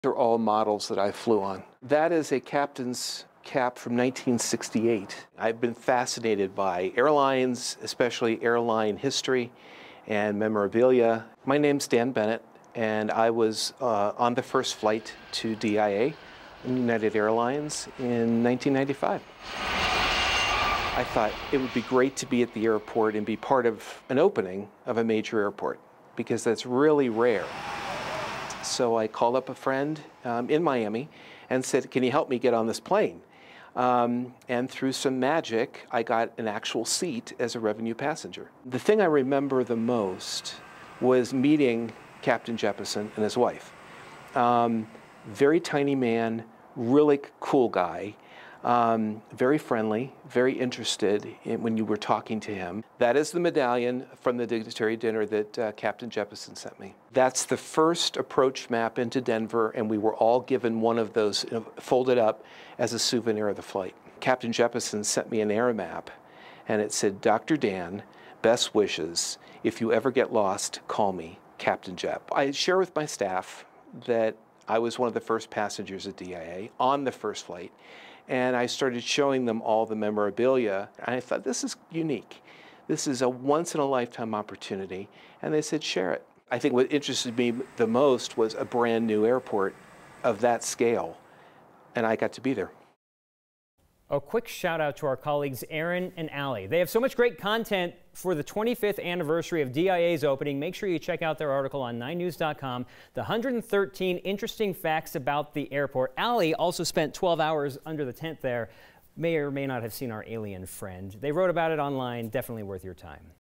An aviation enthusiast talks about being aboard first flight to land at DIA
A doctor from the Denver metro area was aboard the very first flight to land at DIA in 1995.

9NEWS (KUSA) is located in Denver, Colorado.